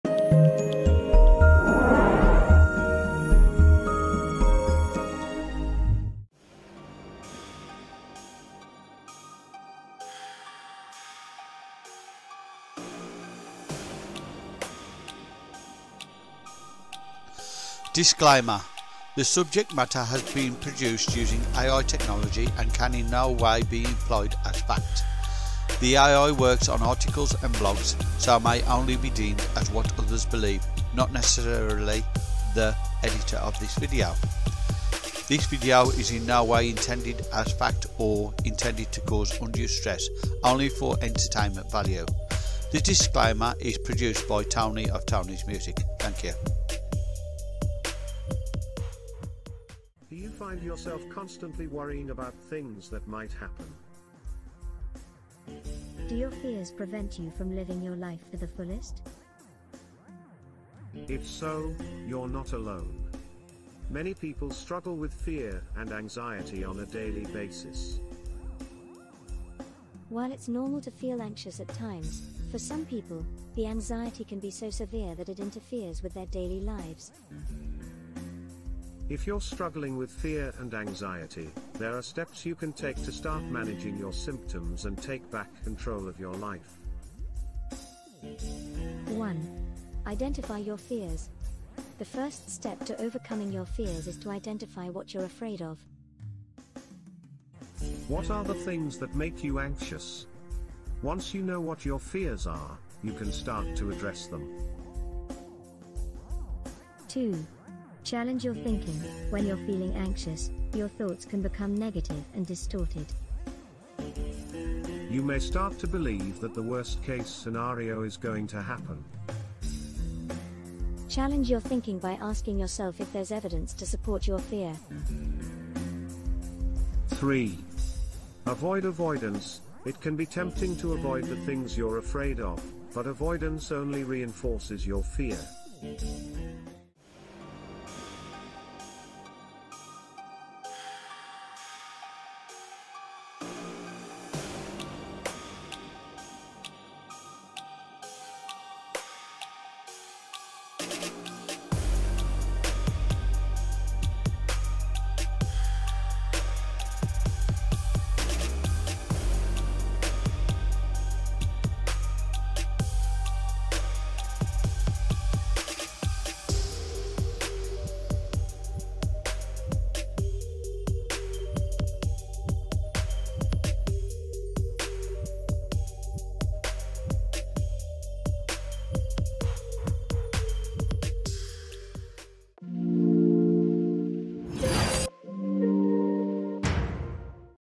Facing Your fears Episode 5 How To Overcome Your Fears
This is the 5th video in the series on facing your fears this one is about how to overcome them!